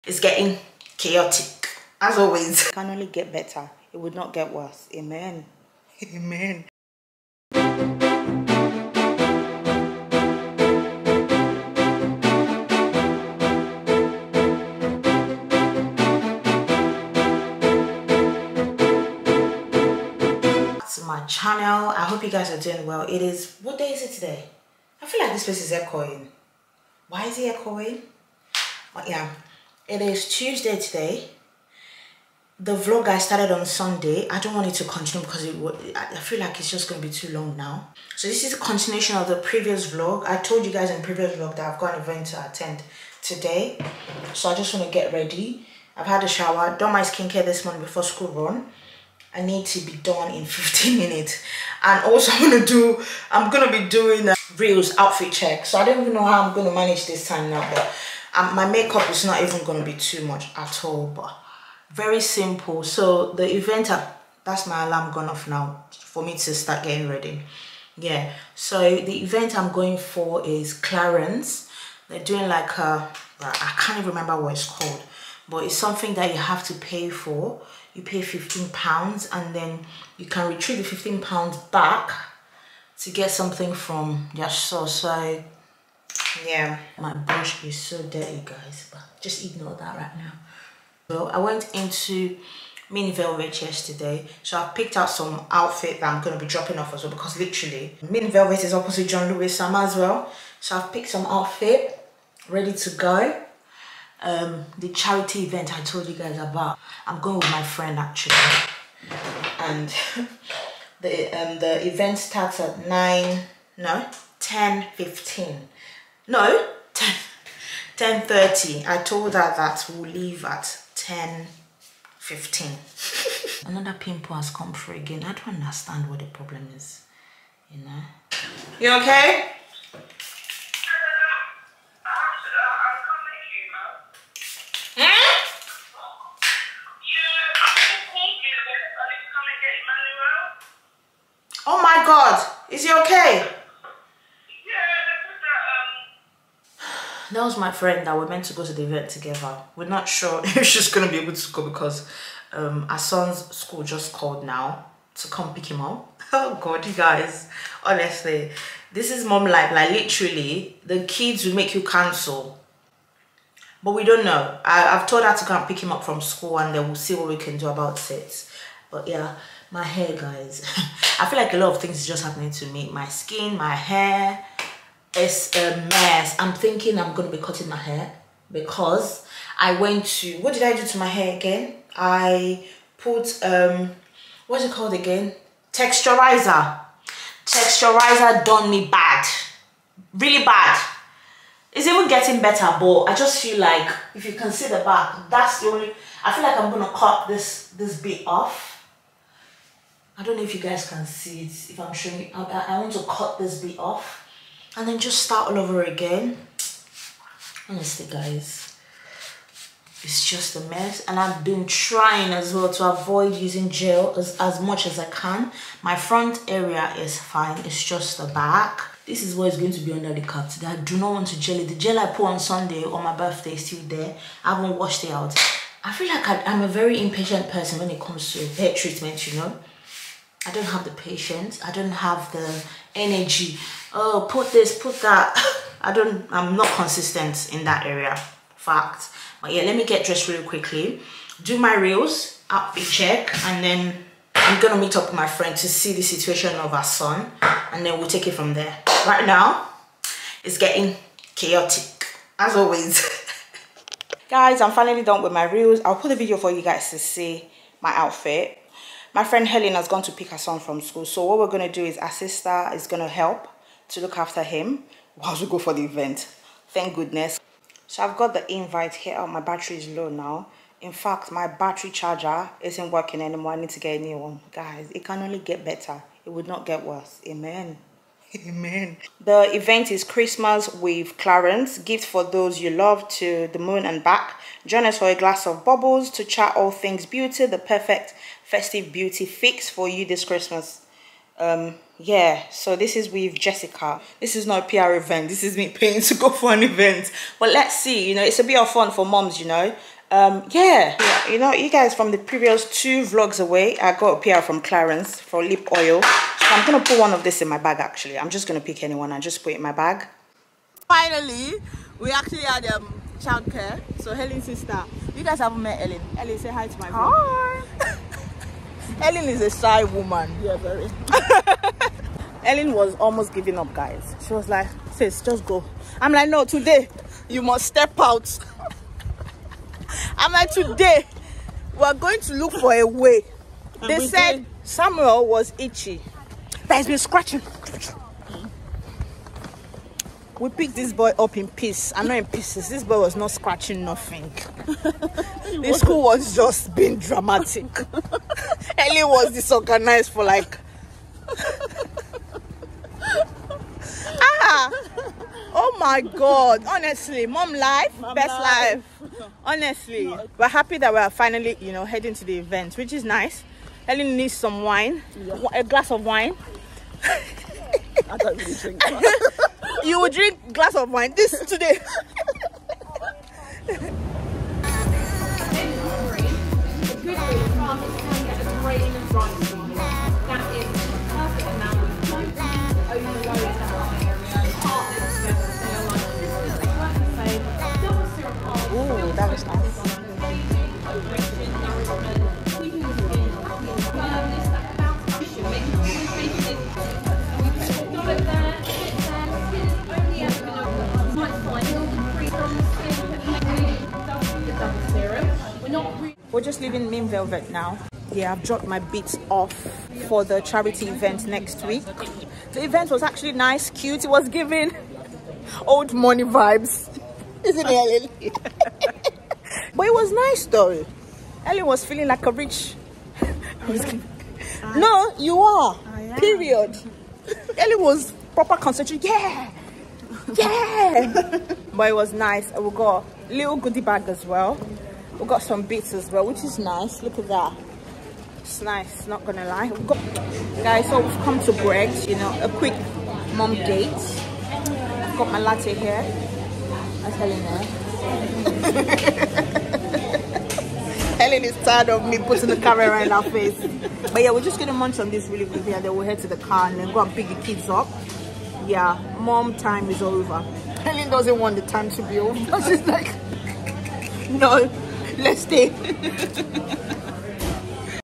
UK LIVING VLOG: HOW CHILDCARE CHAOS & HAIR LOSS ALMOST RUINED MY CLARINS CHRISTMAS EVENT| EP 4
EP 4 VLOG: HOW CHILDCARE CHAOS & HAIR LOSS ALMOST RUINED MY CLARINS CHRISTMAS EVENT| ADEOLA DOMINIC